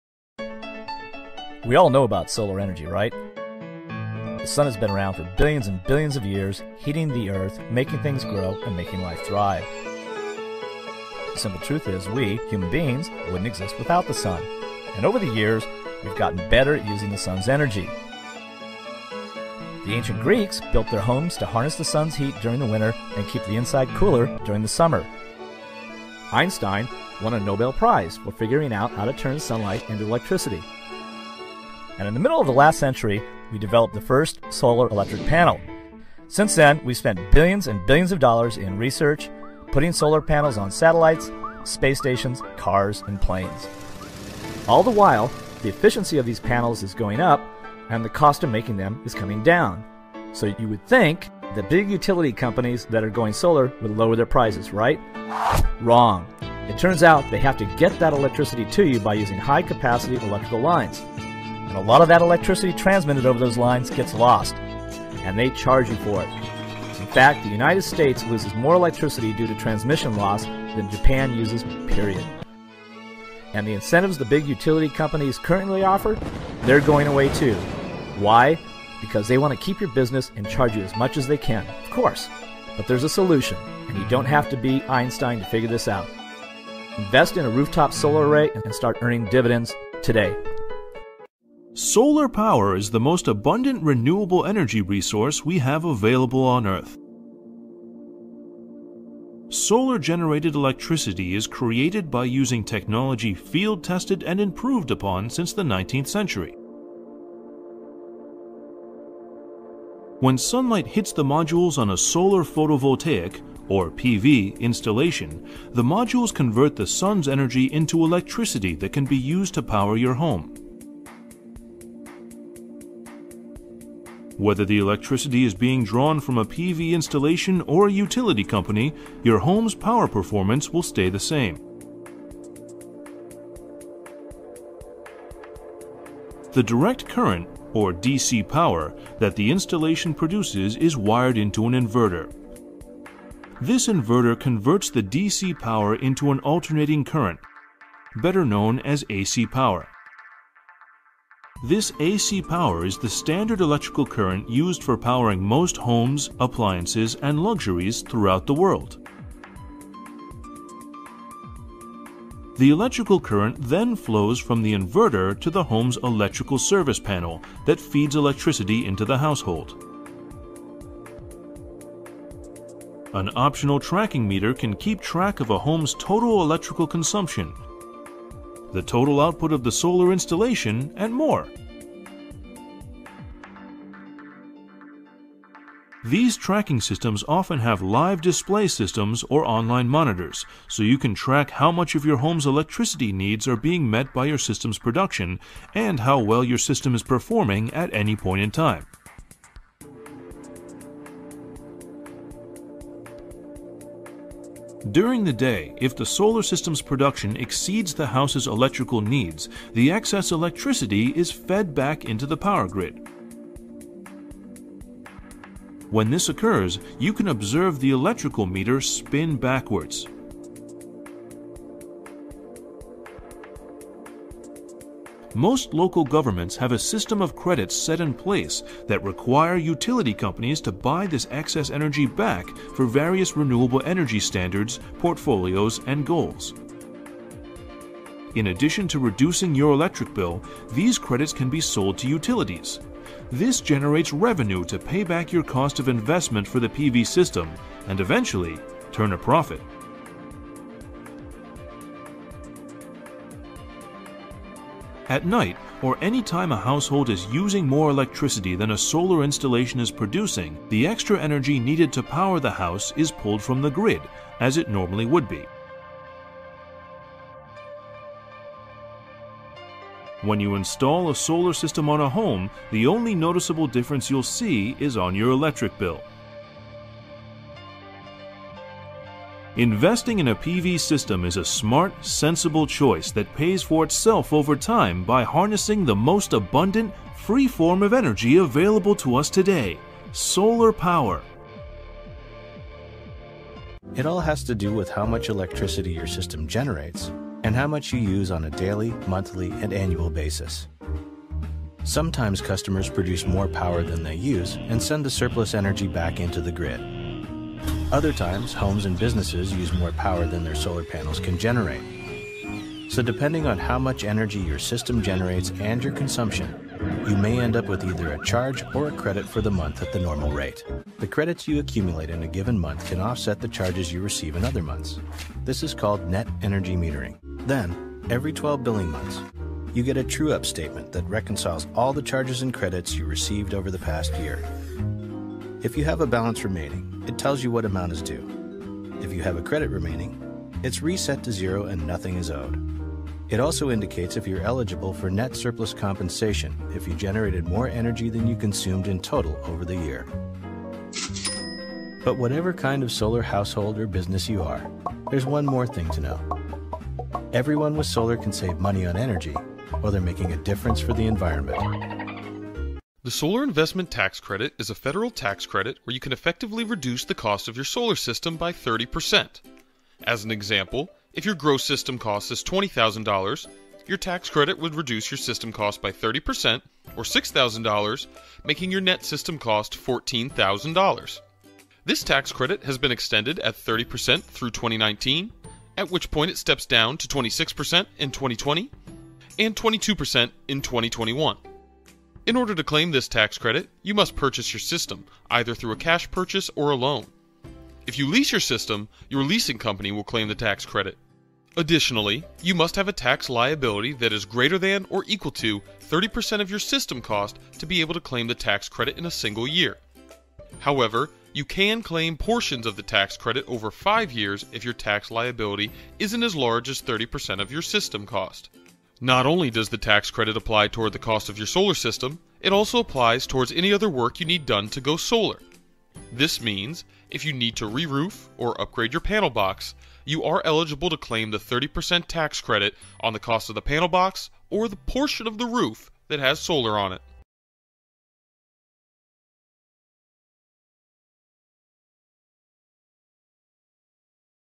Home Solar 101 Need To Know Video. Tom Semark Let the sun power your life!
Learn how home solar energy works and the great benefits of producing your own solar energy.  Please let me know how I can help!  Thank You!  Tom Semark